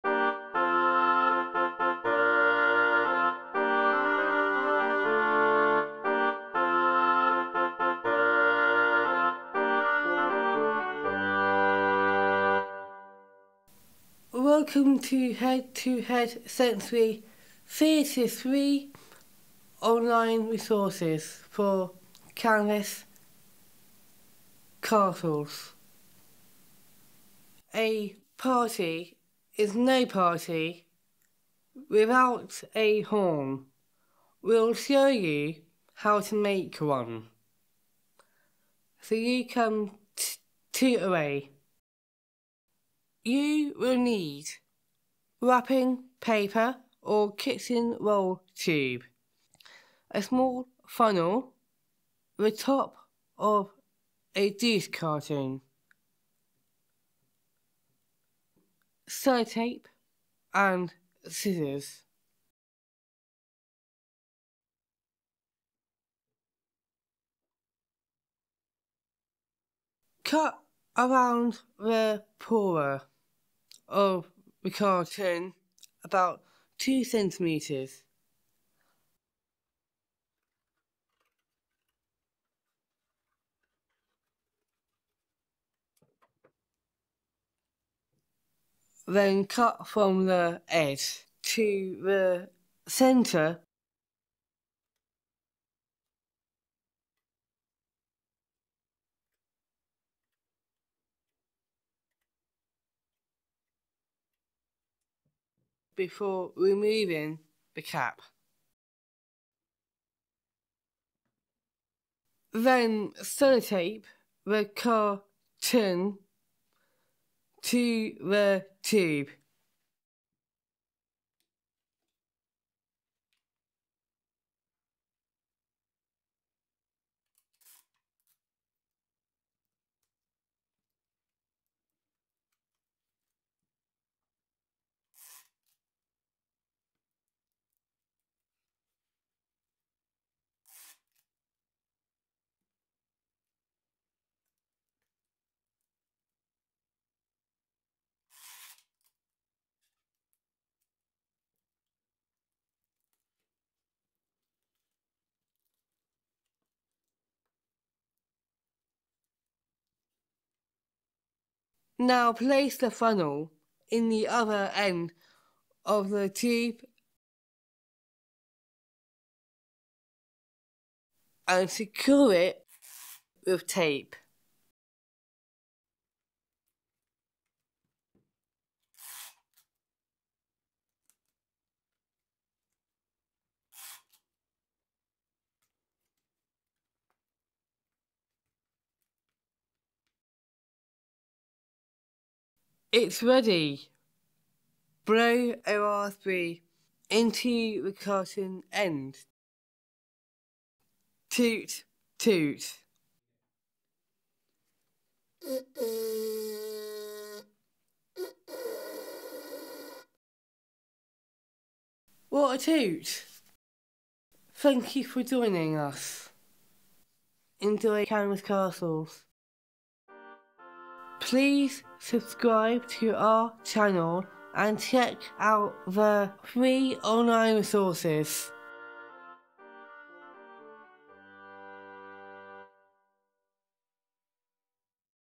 How to make a Horn
Our Canvas Castles You Tube presenter, George, is here to introduce you to the free online resources for Head2Head Sensory Theatre's new interactive and multi-sensory film, Canvas Castles.

If you are heading to the greatest party of all time you'll need a horn, so you can 'toot' away! Join us as we show you how to make your very own horn.